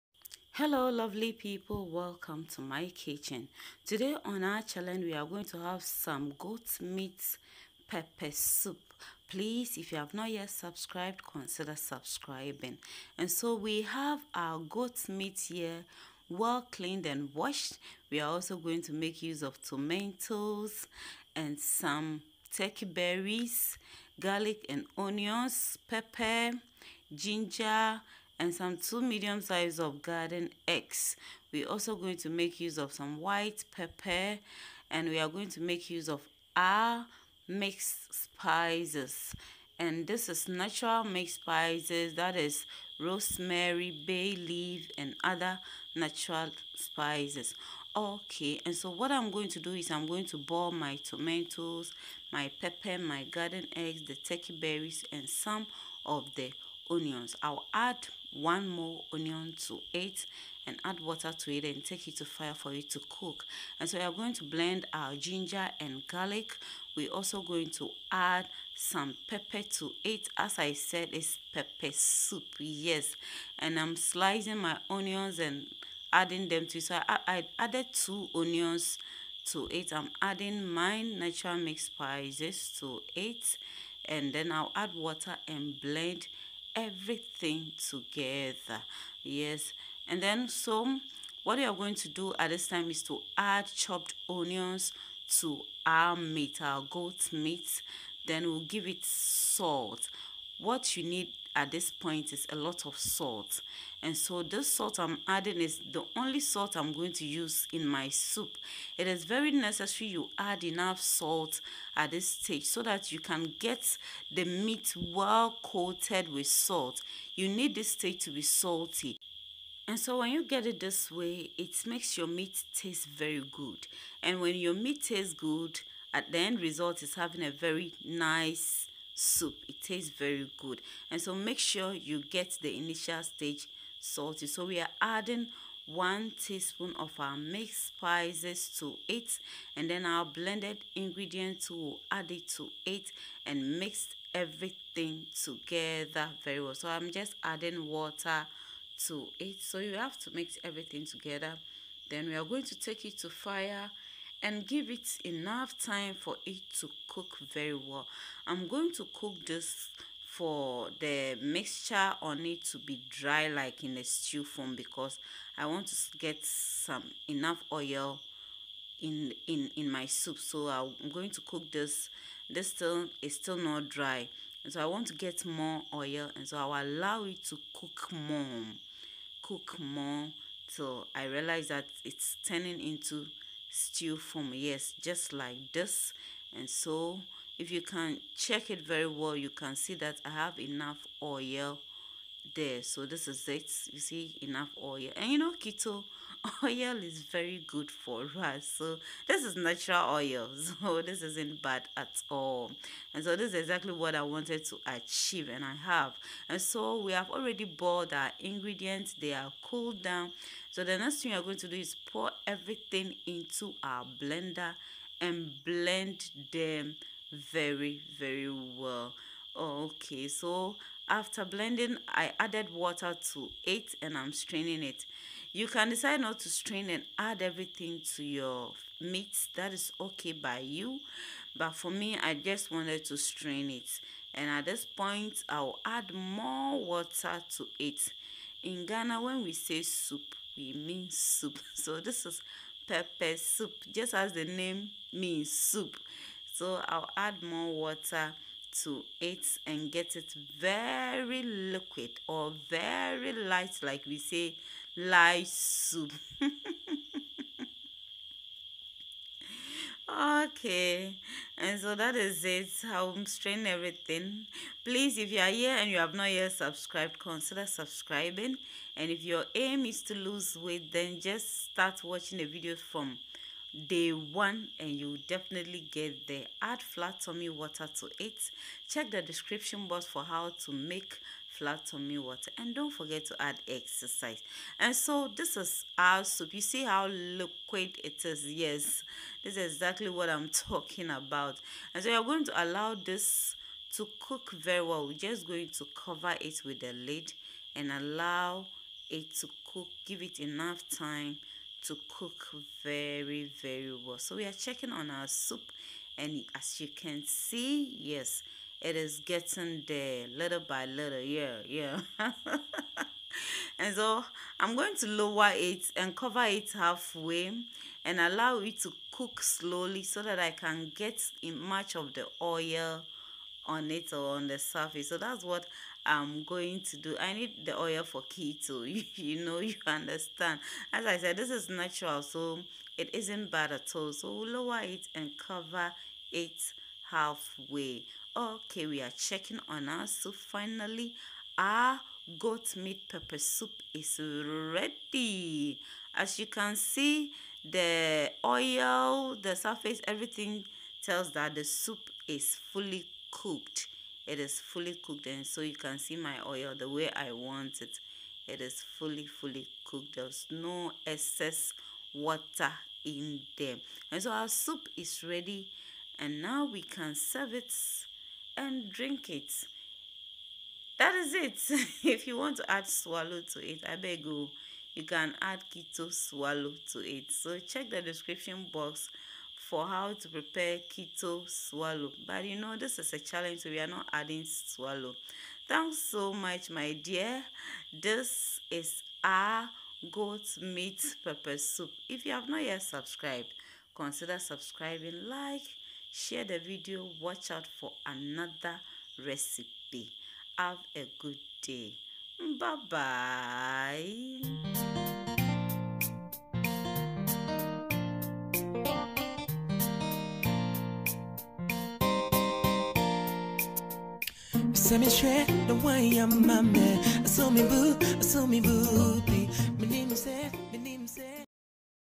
Weight Loss Challenge| Day 22| Goat 🐐 Meat 🍖 Pepper 🌶️ Soup 🍲| OMAD Challenge| Ketogenic Lifestyle
Goat meat
tomatoes
onions
Pepper
garlic
ginger
spices
garden eggs
turkey berries
salt